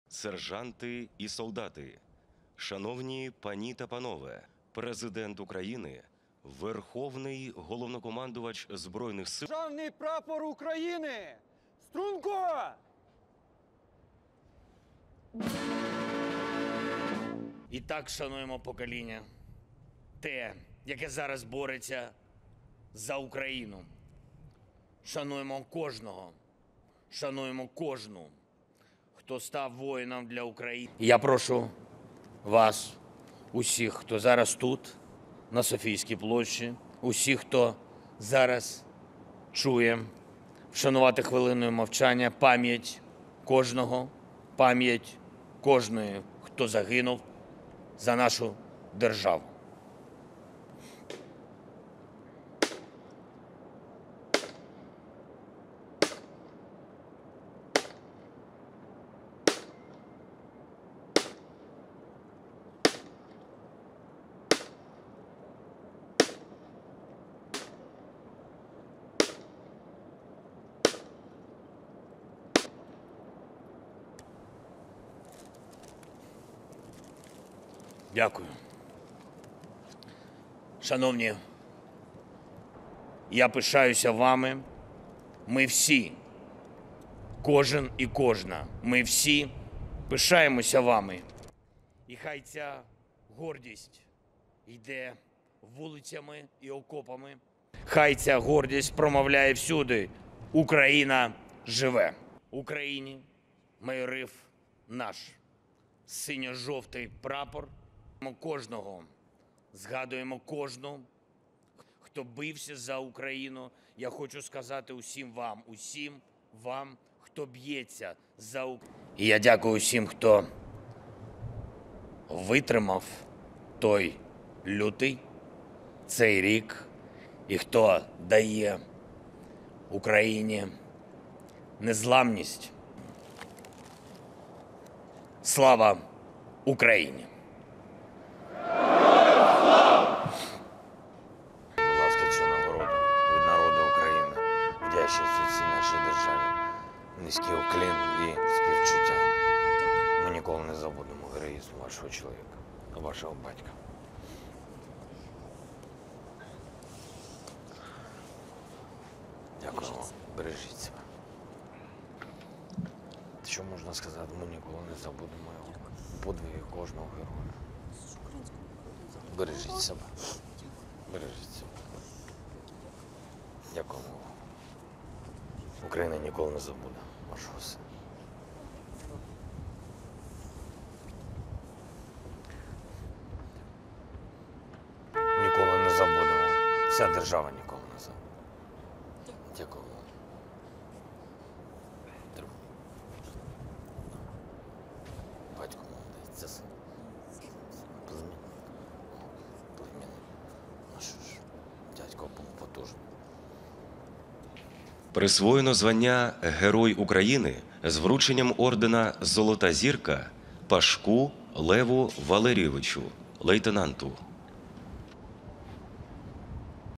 'Ukraine is Alive’: Volodymyr Zelenskyy
Ukrainian President Volodymyr Zelenskyy marked the first anniversary of Russia's full-scale invasion on Friday  with a moment of silence and words of gratitude. The 45-year-old addressed members of Ukraine's armed forces and a small gathering of dignitaries in St Sophia Square, next to the green- and gold-domed cathedral that is a symbol of the city's resilience.

“May this be proudly proclaimed everywhere: Ukraine is alive,” Zelenskyy said.

A year ago today, Russian President Vladimir Putin launched a “special military operation” in Ukraine blaming the escalation of the conflict on Ukraine’s pivot to the West.